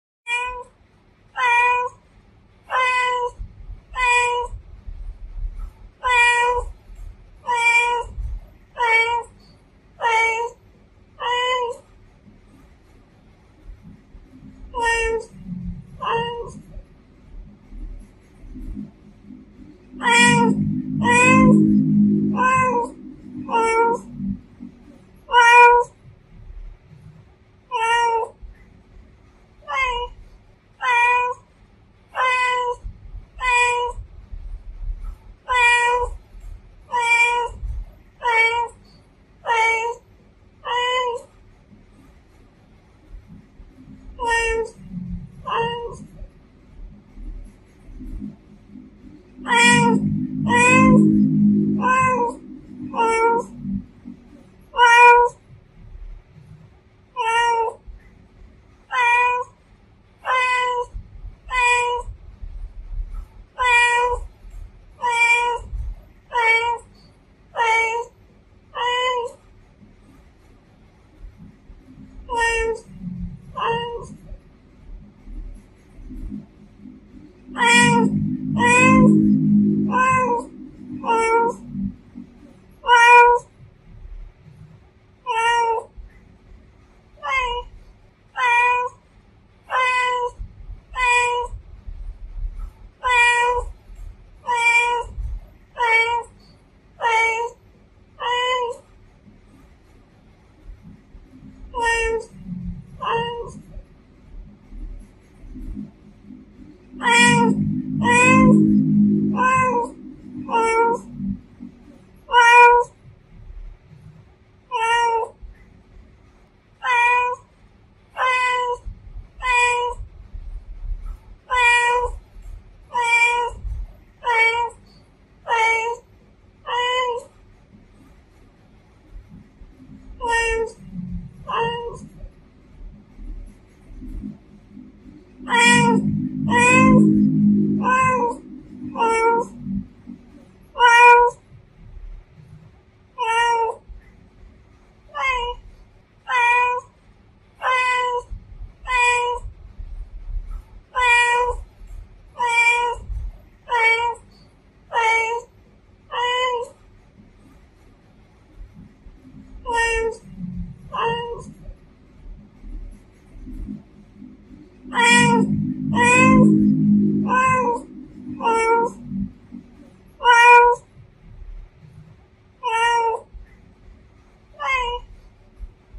FEMALE CAT SOUND - PRANK YOUR PETS
Here is a video of female cat meowing to confuse your pets

Like if you love cats.